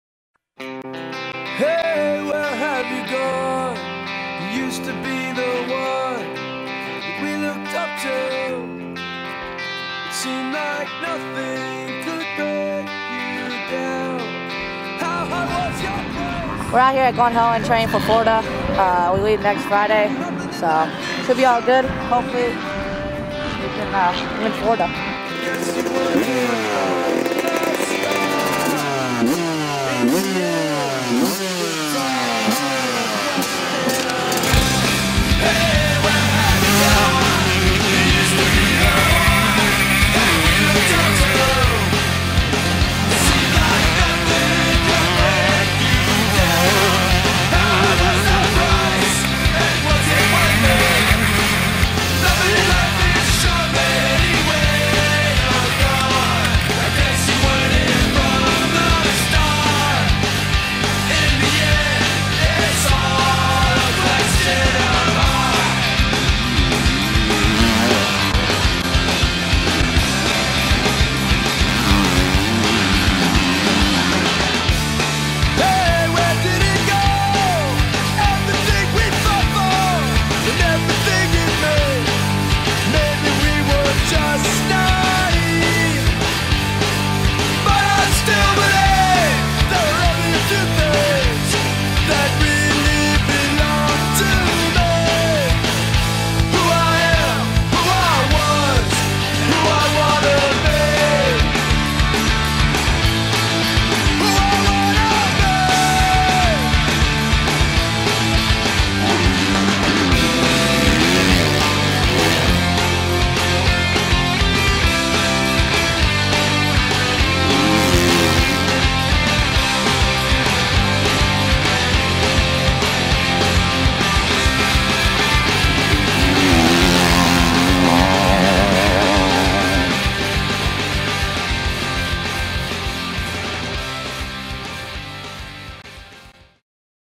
Glen Helen Training for Mini O's ft Owen / Green / Baker / Bisceglia
With The 40th Annual Thor Winter National Olympics  - Mini O's - just right around the corner the teams and riders are finishing up testing and getting the bikes dialed in for the new season and Vurb Moto was there to film them.

Featuring Blake Green / Jace Owen / Daniel Baker and Matt Bisceglia